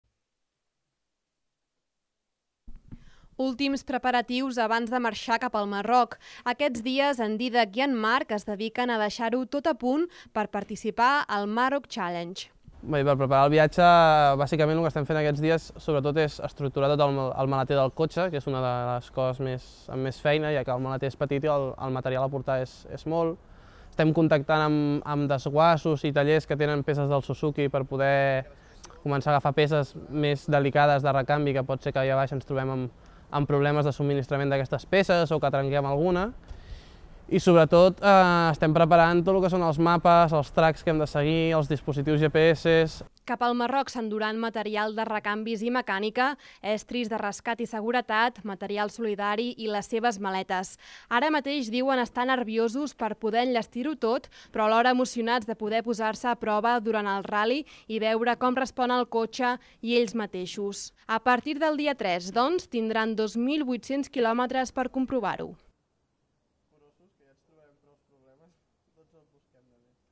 Últims preparatius pel Maroc Challenge
En Dídac i en Marc enllesteixen els últims preparatius de cara al rally solidari que els durà pel desert del Sàhara el proper mes de desembre.